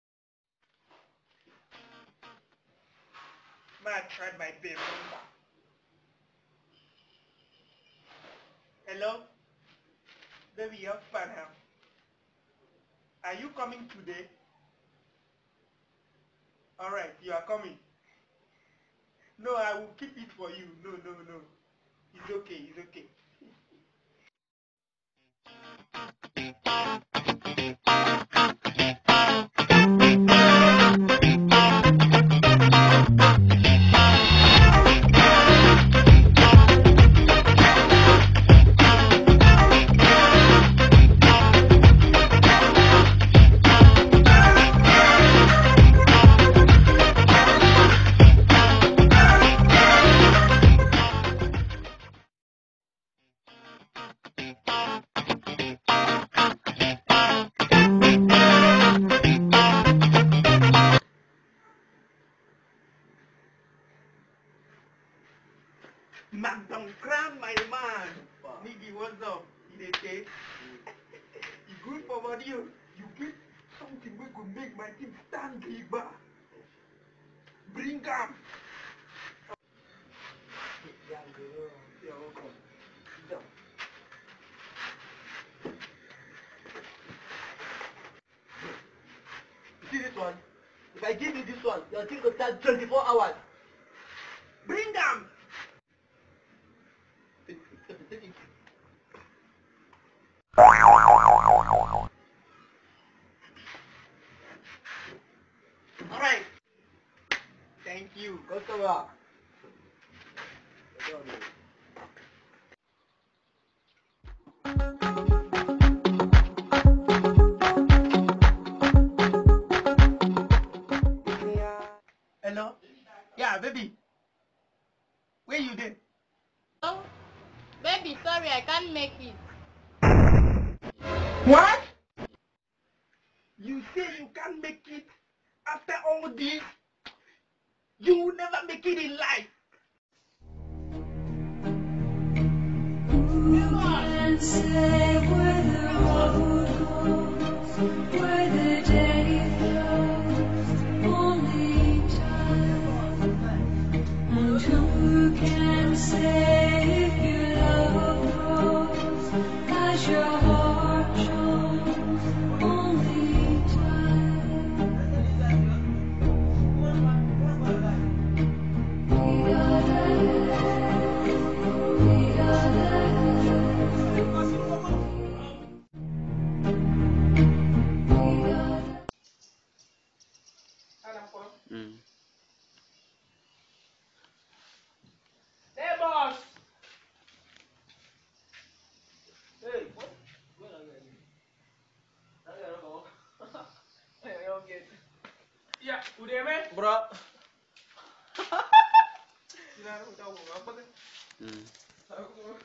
What happen when a girl fail you in naija
after taking all types of manpower she say she is not coming . ..must watch comedy